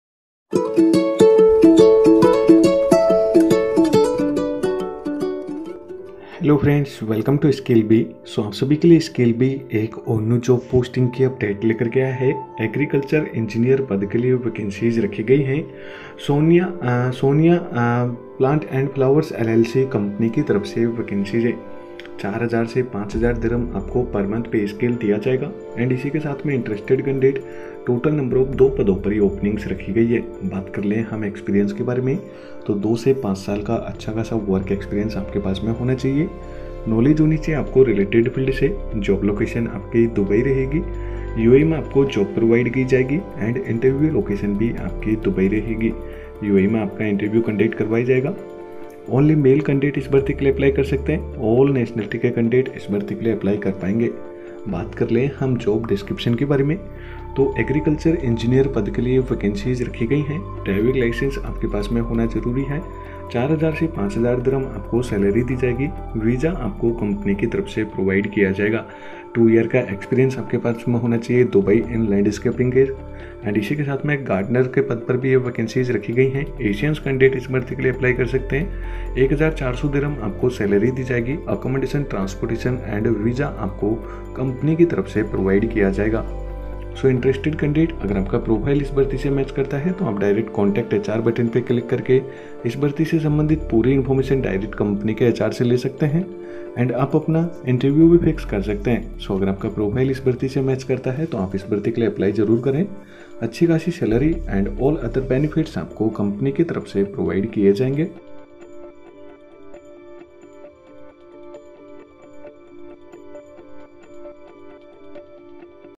JOB IN UAE / Role: Agriculture Engineer / Company: Sonya Plants & Flowers LLC / Salay: AED4000-5000
15. Check out this job opening on the Skillbee app that is available for you:

Role: Agriculture Engineer

Company: Sonya Plants & Flowers LLC

Openings: 2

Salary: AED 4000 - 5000

Experience: 2 - 5 years

Job Location: Dubai - United Arab Emirates

Interview Location: Dubai - United Arab Emirates